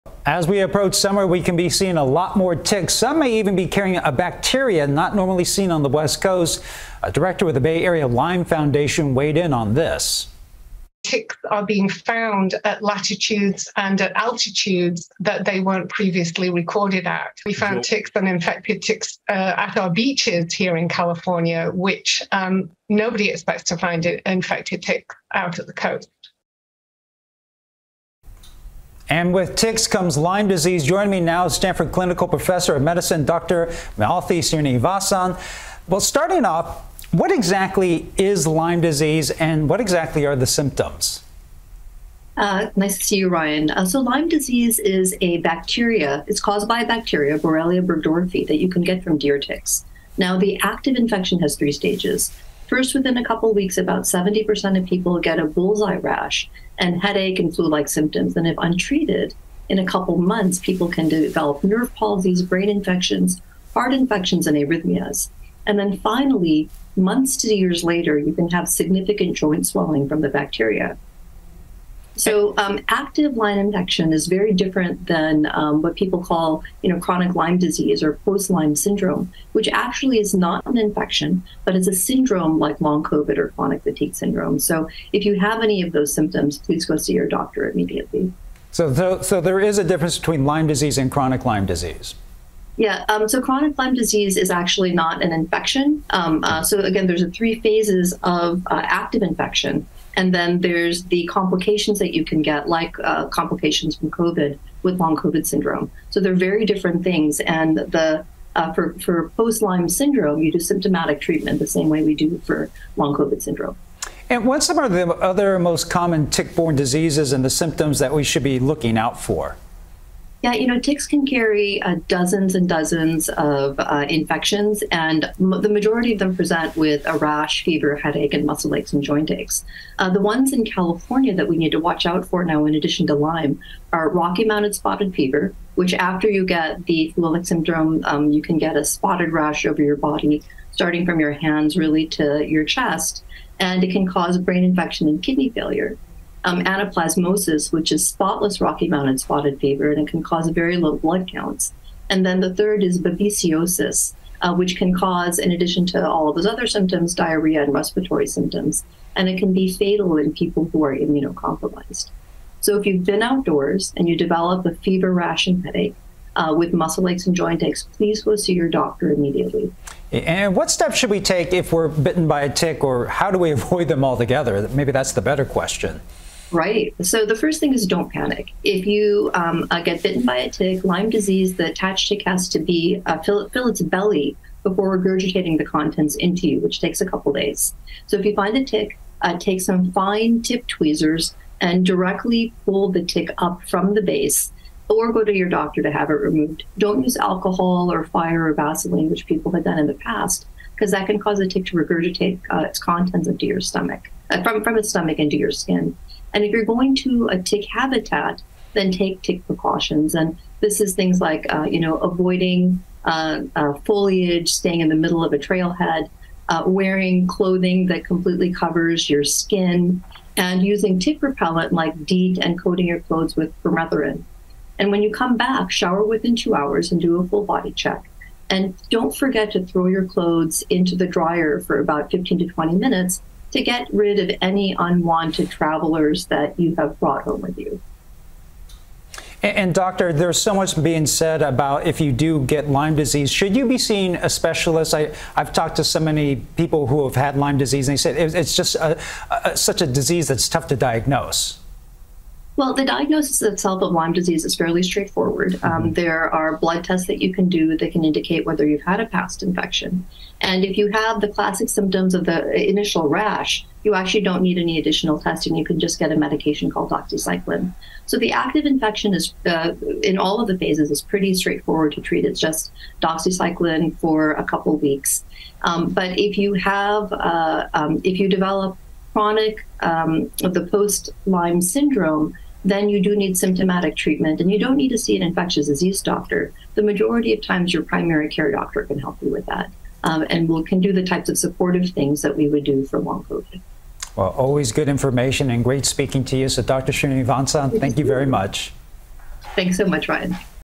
Tick season brings threat of Lyme disease
CBS News Bay Area anchor Ryan Yamamoto asks Dr. Malathi Srinivasan, Clinical Professor of Medicine at Stanford, about the symptoms of Lyme disease, and some of the other most common tick-borne illnesses